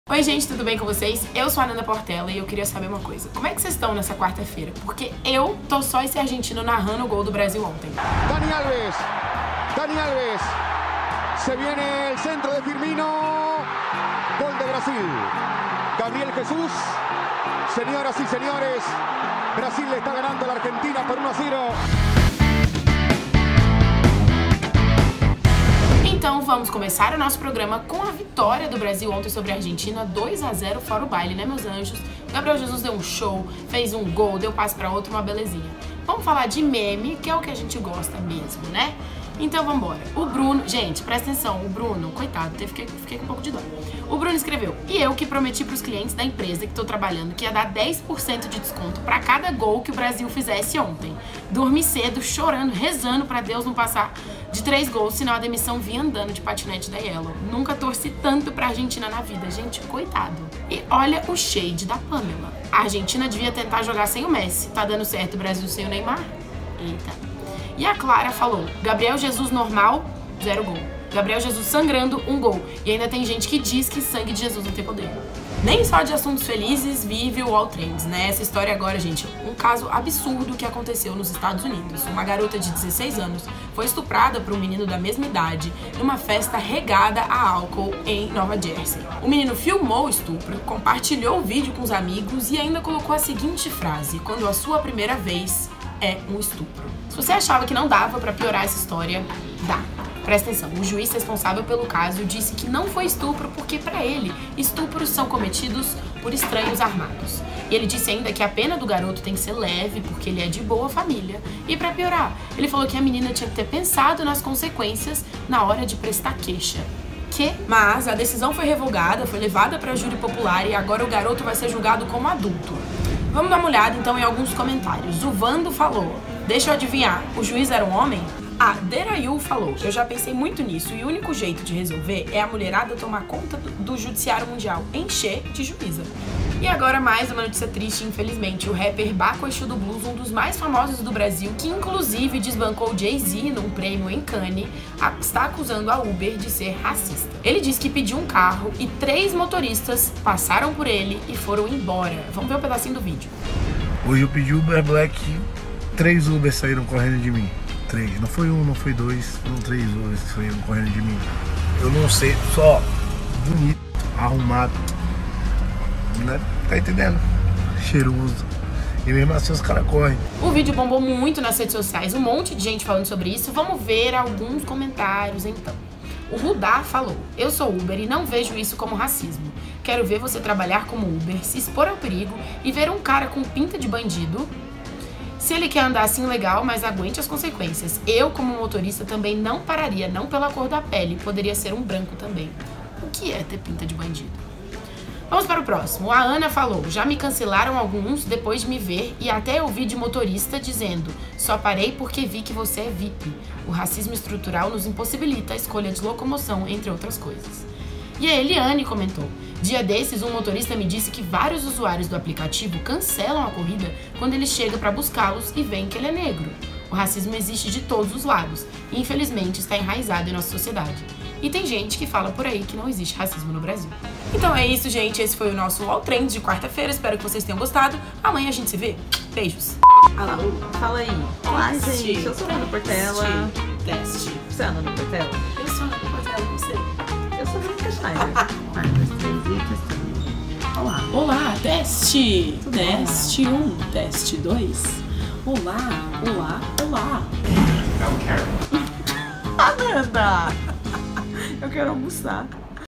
UOL TRENDS: NARRADOR ARGENTINO CHATEADÍSSIMO
A animação de vocês está igual a do narrador argentino na vitória do Brasil? Este é um dos assuntos do UOL Trends, o programa que te conta tudo que está bombando por aí!
Apresentação: Ananda Portela
Produção: Ananda Portela, Flora Paul e Veronica Schneider
Edição: Paulo Castro